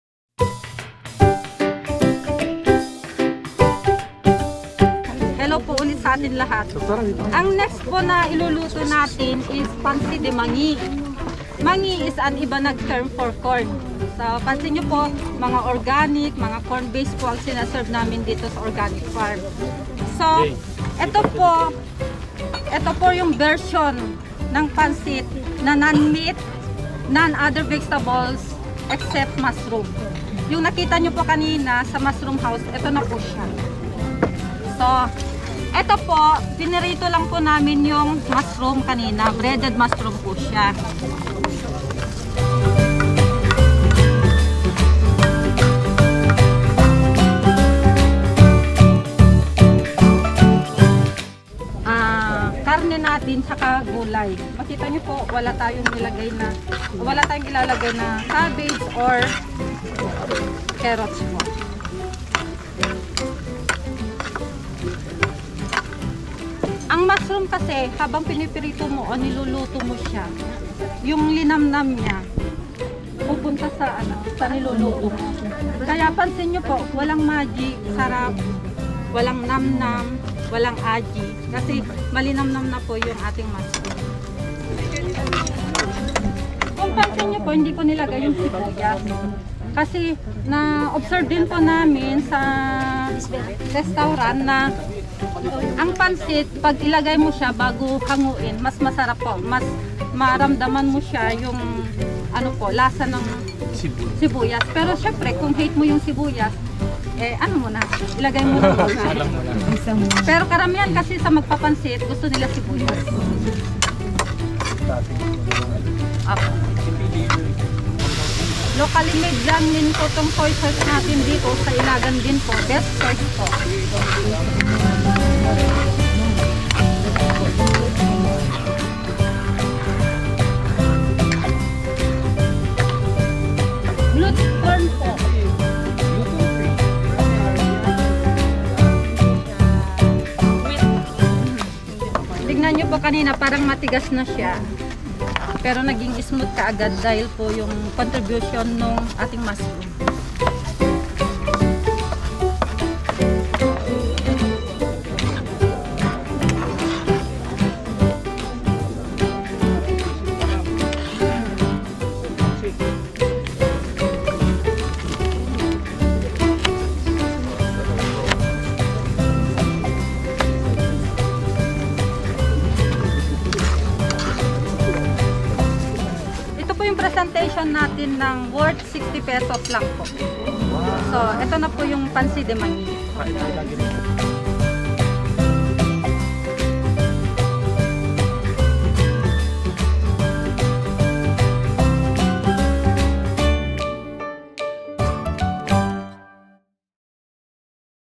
PMT | ILAGAN CITY PANSI DE MANGI COOKING DEMO
Slow Food demonstration featuring the Pansi De Mangi with Fresh Oyster Mushroom at the Department of Agriculture Cagayan Valley Research Center Agro Eco-Tourism Farm at Ilagan City, Isabela as part of the Philippine Motorcycle Tour Caravan as a way to promote Region 02 culinary products.